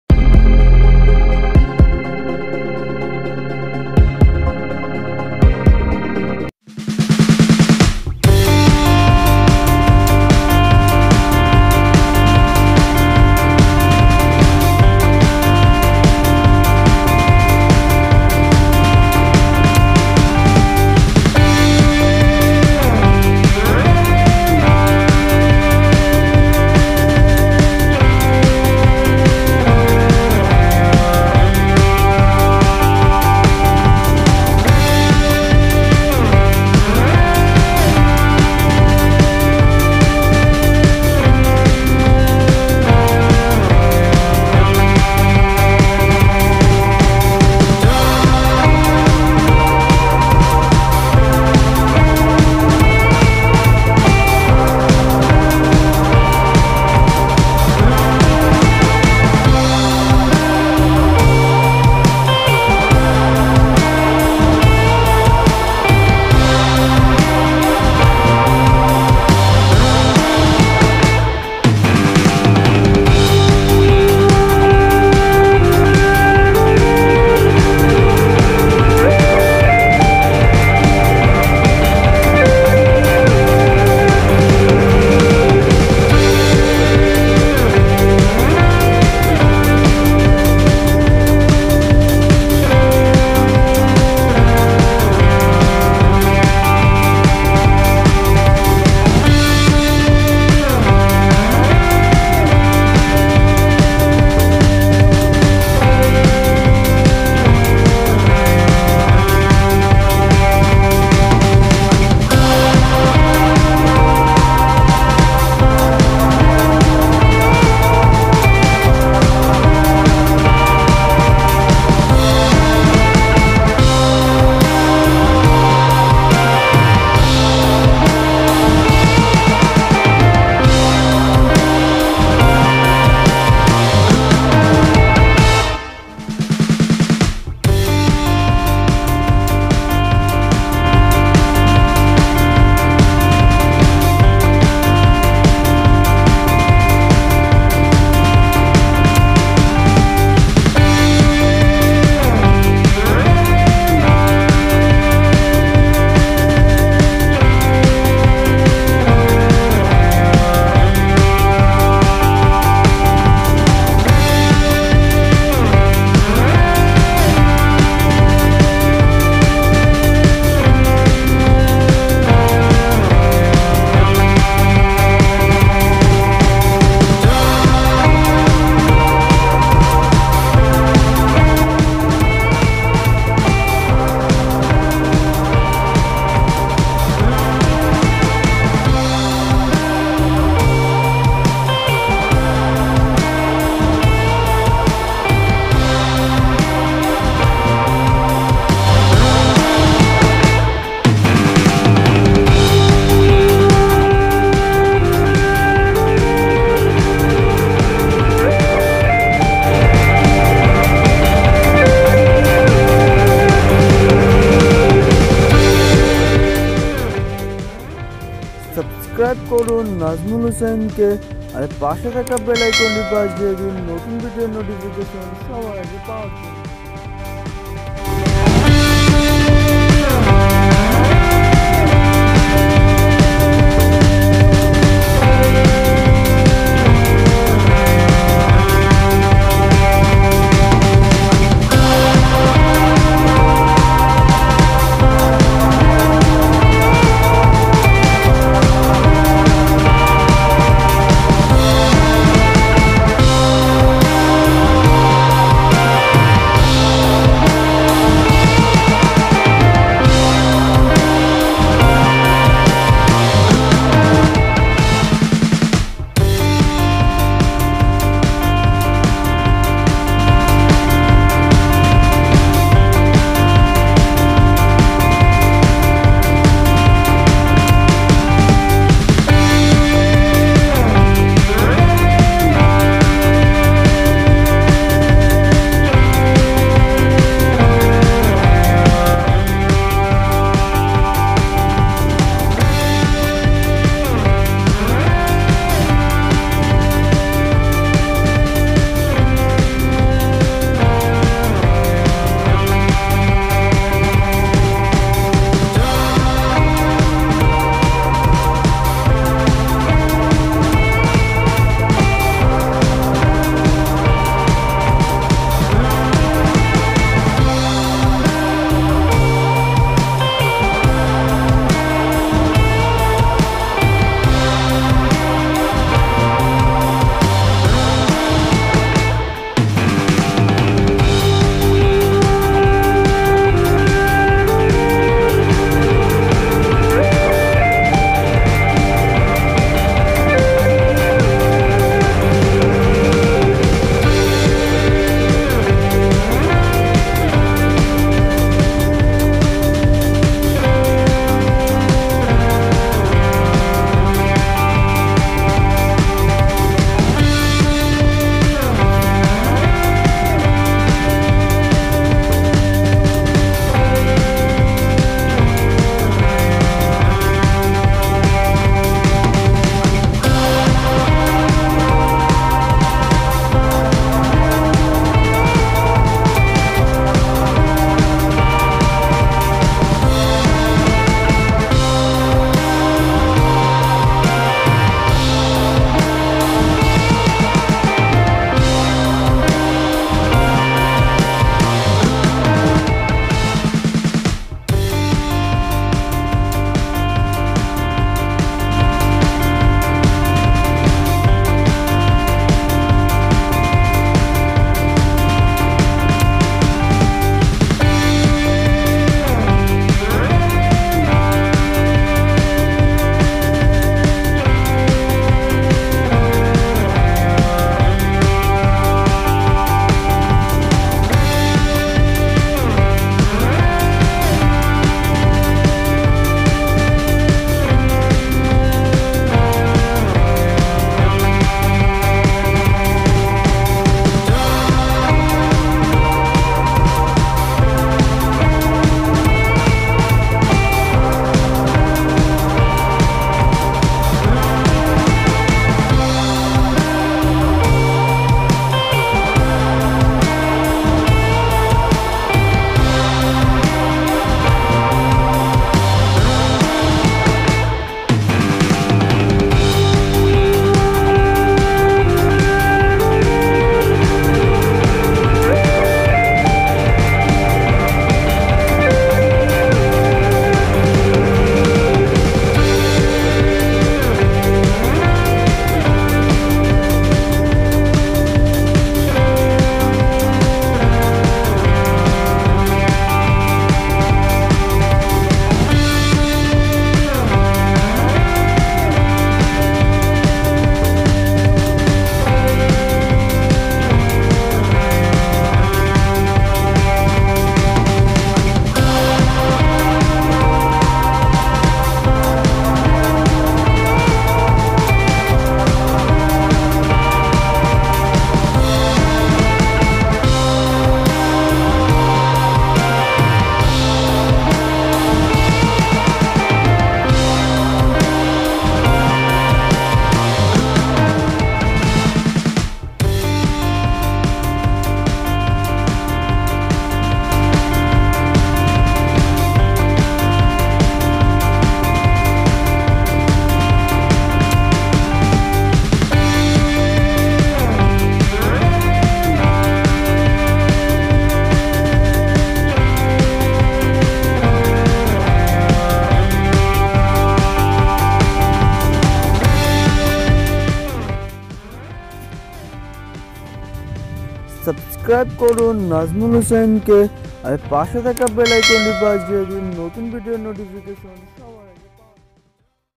Best Photo Edit by Mobile Phone. Photo Edit Bangla Tutorial.. Nazmul Hossain.
How to Edit Awesome photos in Mobile Bangla Tutorial.. In this tutorial you see and and you can make awesome photos for your website..

Nazmul Hossain
Nazmul Hossain
Nazmul Hossain
Nazmul Hossain